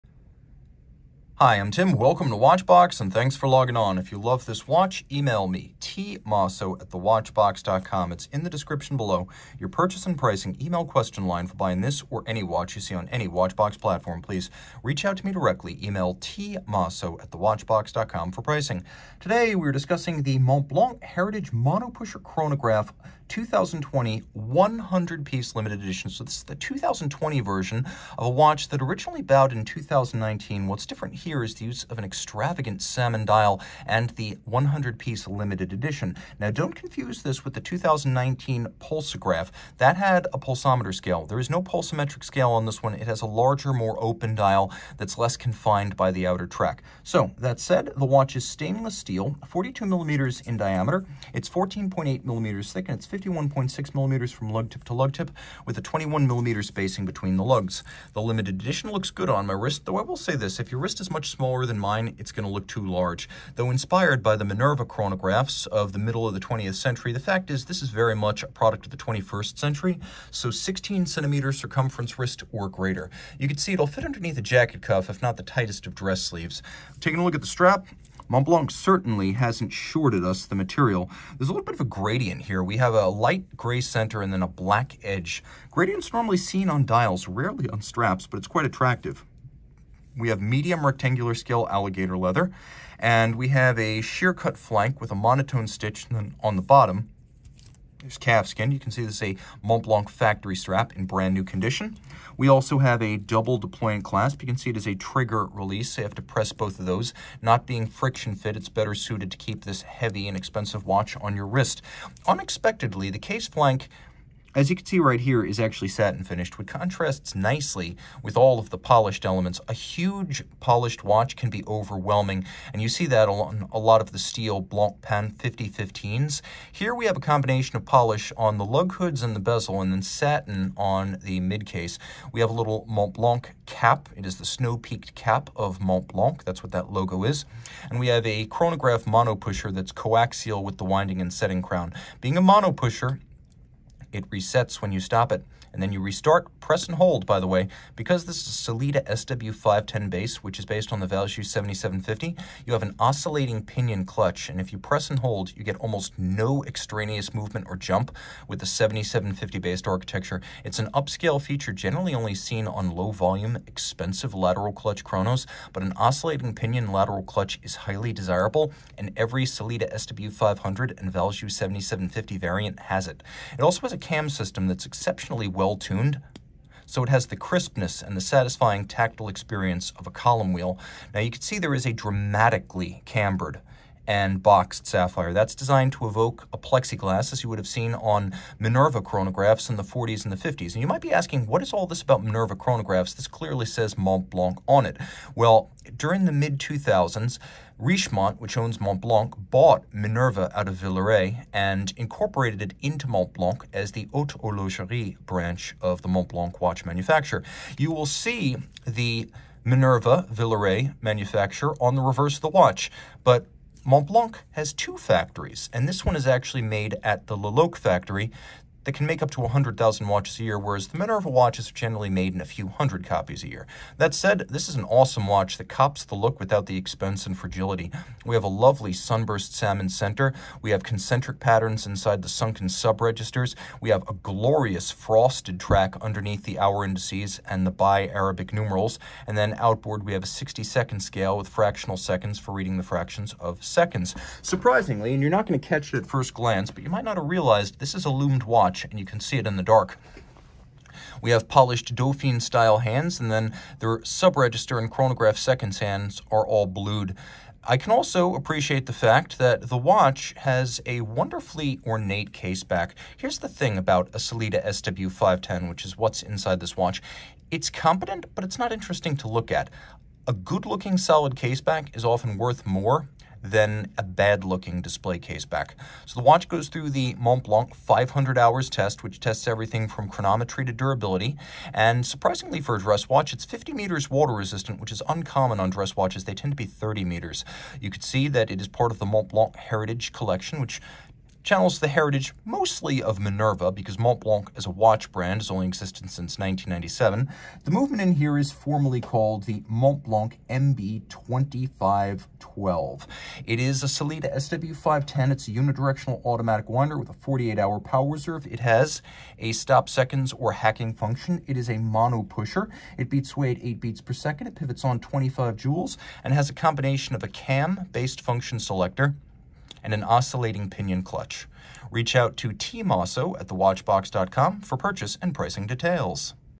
Montblanc Heritage Monopusher Chronograph 126078 Montblanc Watch Review
SKU: 4628509

The Montblanc Heritage Monopusher Chronograph 126078 is encased in 42mm of stainless steel surrounding a salmon dial on a grey alligator strap. Features of this Montblanc Heritage Monopusher Chronograph include hours, minutes, small seconds, and chronograph.

For complete details, watch the full Montblanc Heritage Monopusher Chronograph review.